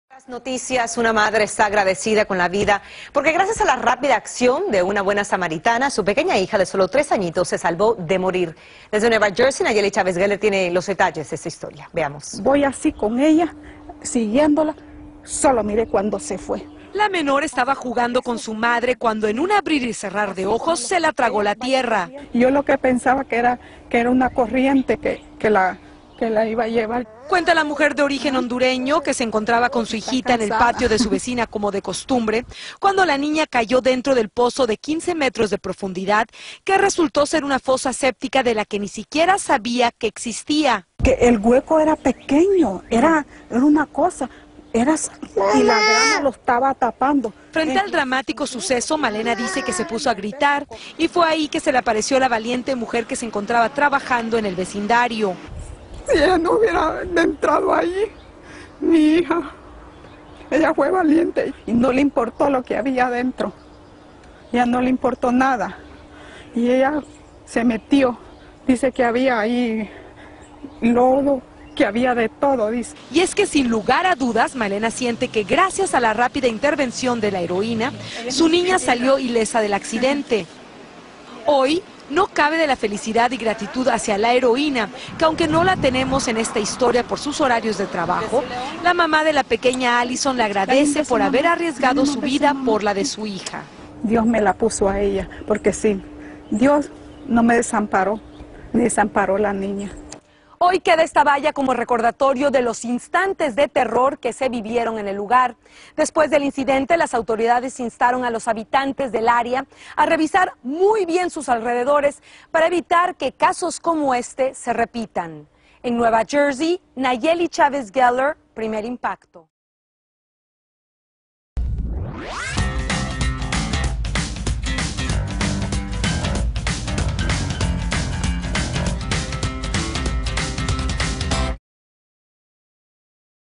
La buena acción de una samaritana le salvó la vida a una pequeña
Nayeli Chavez reporta desde Nueva Jersey como la buena acción de una samaritana le salvó la vida a una niña.